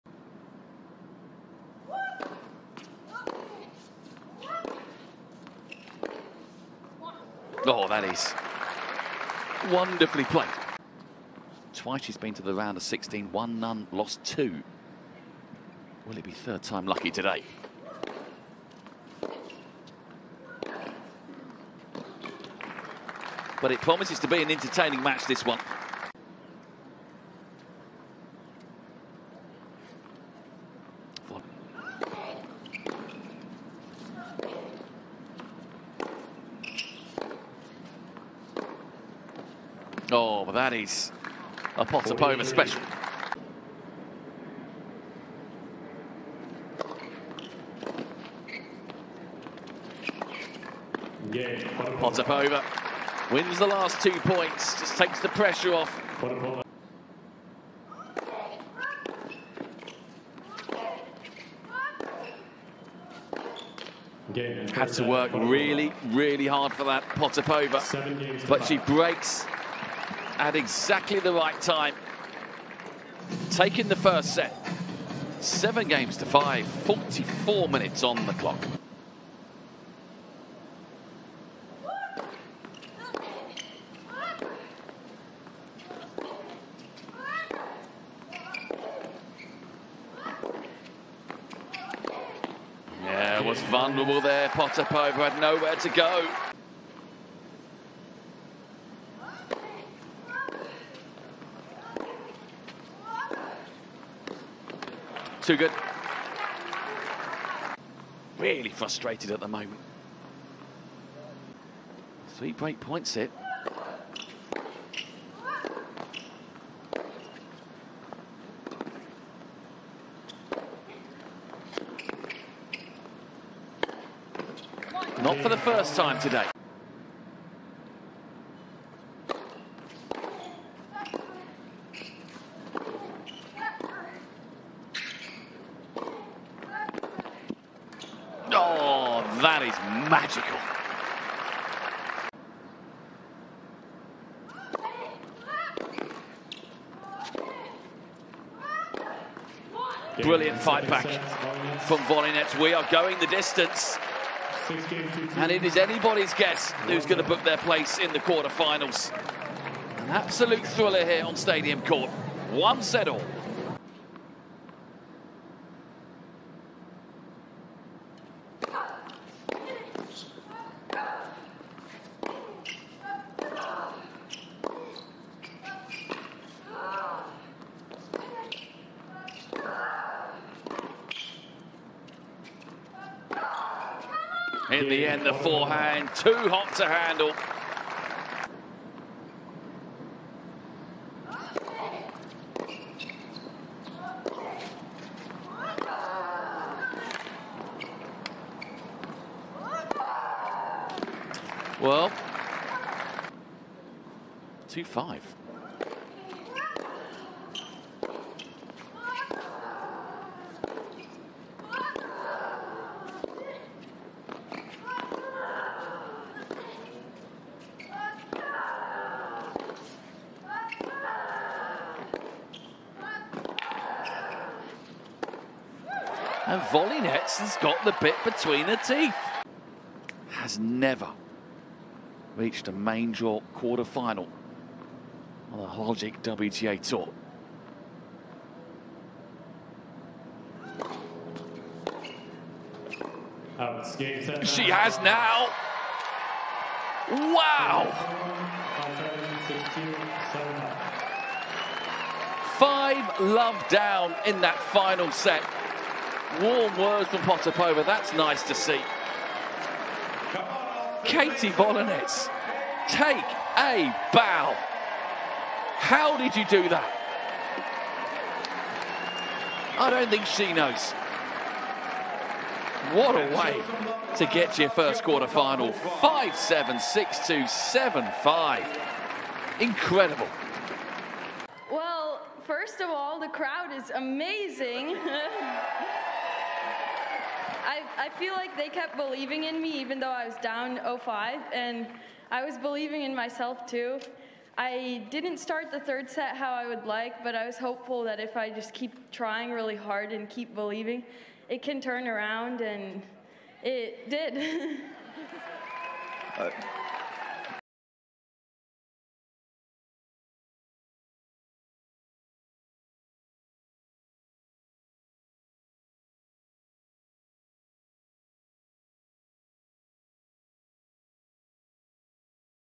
Anastasia Potapova vs. Katie Volynets | 2023 Austin Round of 16 | WTA Match Highlights
Watch the Match Highlights between Anastasia Potapova vs. Katie Volynets at the 2023 ATX Open in Austin.